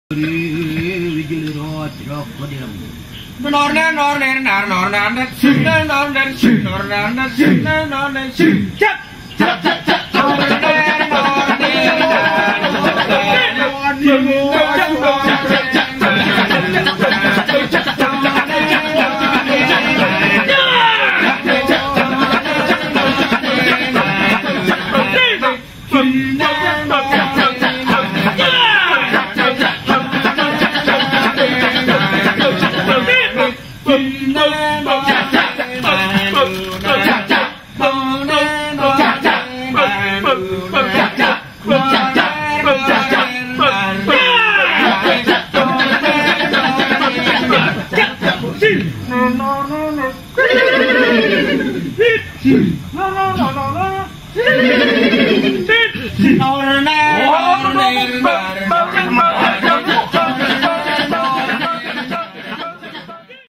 Monkey Riding - August 2011
Back in 2011, fixed gear bicycles in Bali and a traditional Balinese kecak dance.